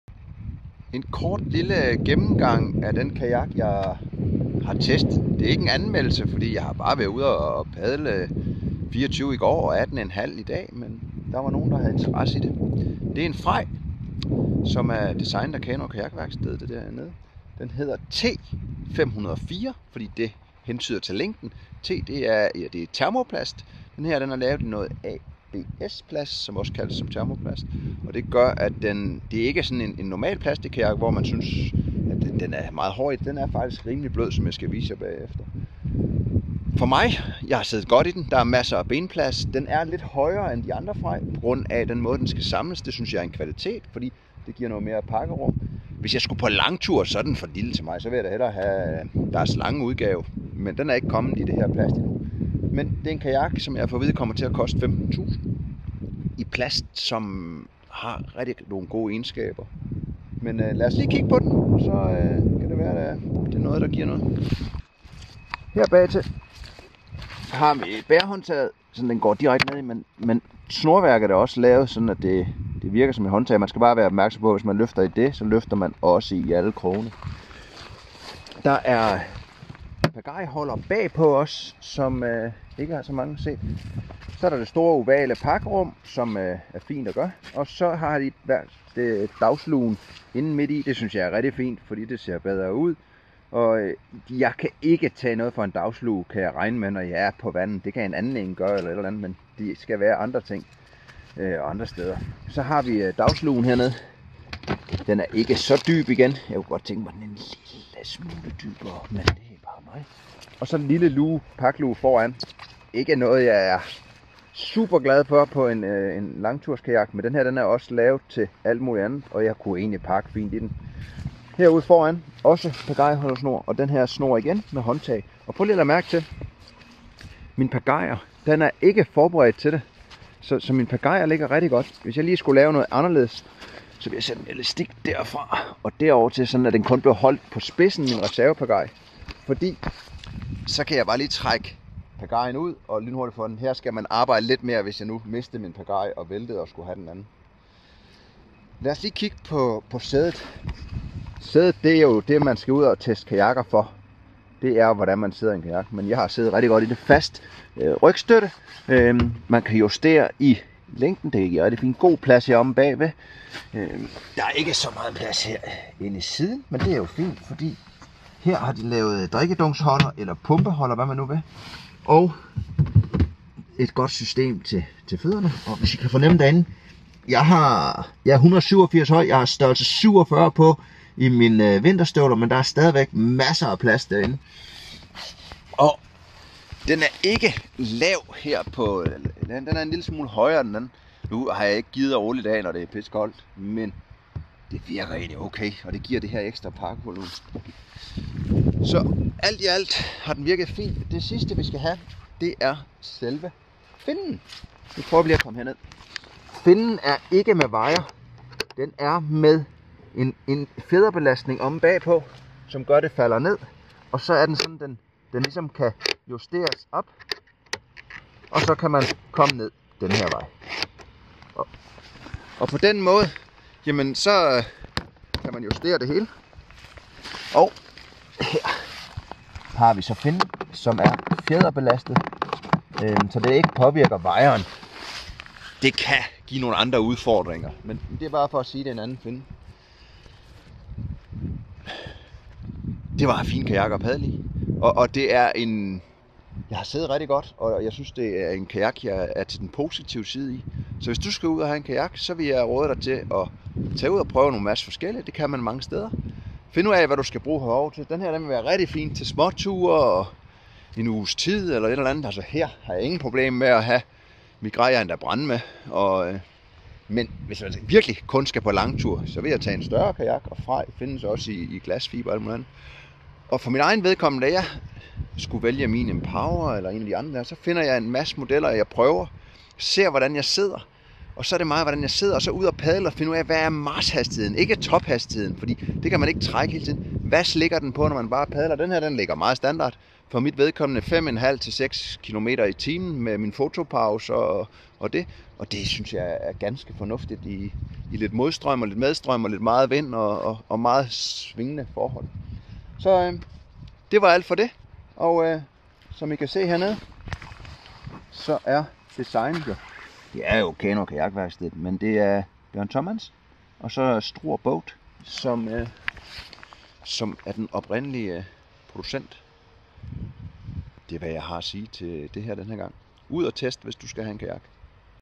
Lidt om FREJ T504
Jeg gennemgår kort havkajakken FREJ T504, Struer Boat, efter en prøve tur i Roskilde Fjord over 2 etaper, en på 24 km og en på 18,5 km.
Du kan også se en video og beskrivelser af selve turen her
Erik B. Jørgensen